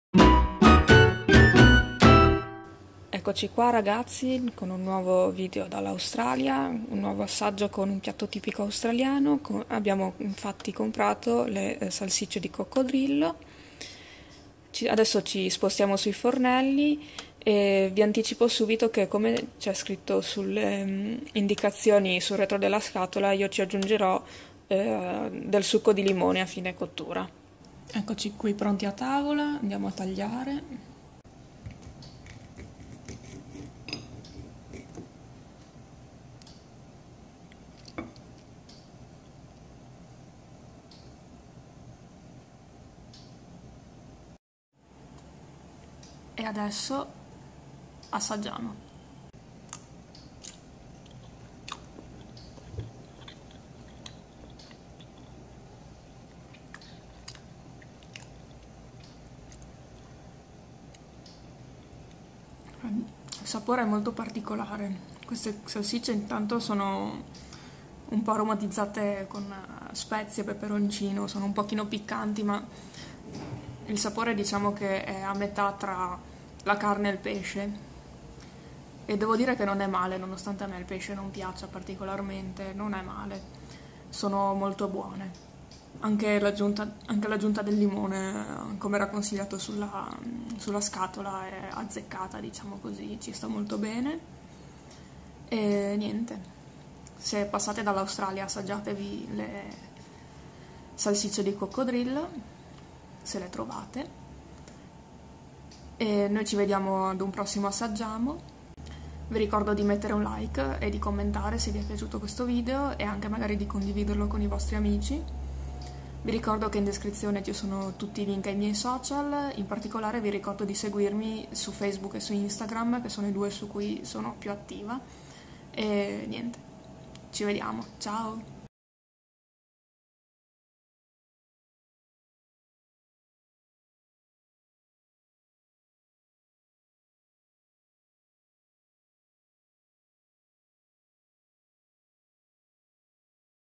Crocodile Sausage 🐊 assaggiAMO!
Crocodile Sausage... saranno buone le salsicce di coccodrillo?